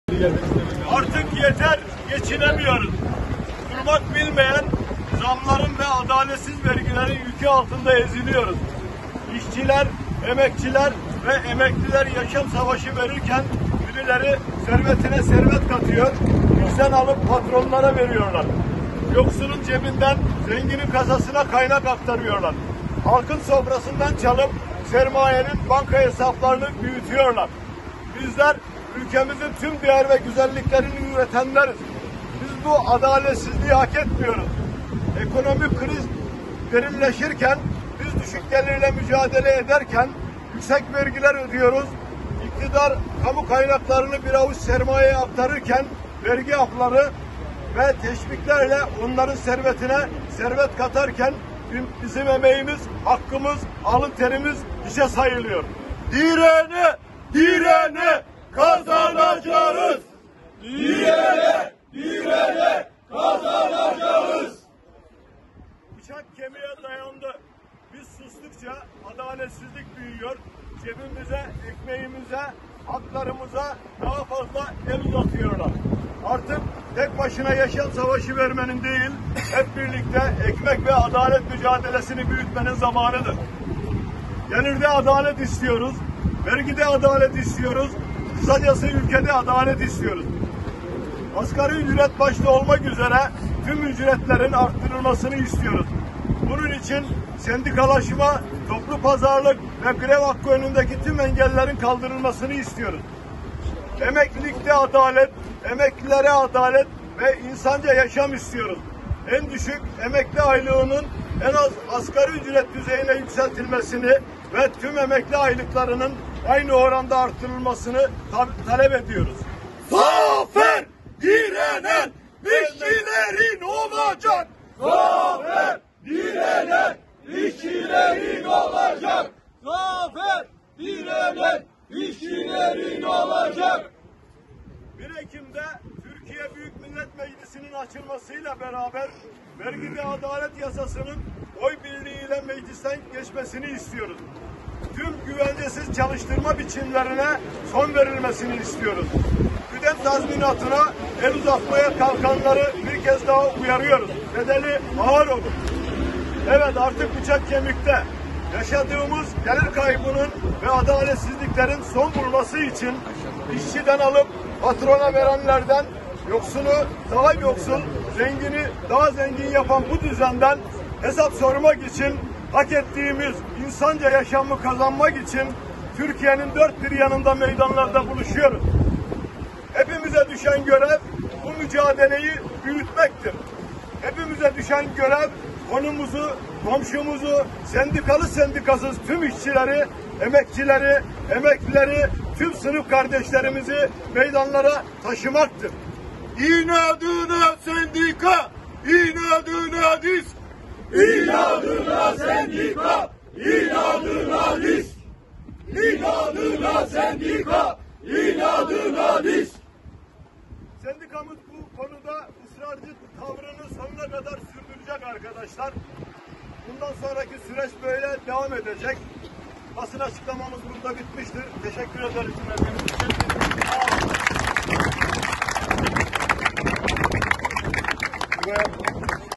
ARTIK YETER! GEÇİNEMİYORUZ! AÇIKLAMASI
Prysmian işçileri, DİSK'e bağlı sendikalar ile ortak olarak yaptıkları açıklamalarında gelirde adalet ve vergi yüklerinin altında ezildiklerini ifade ederek yaşam savaşı verdiklerini söylediler.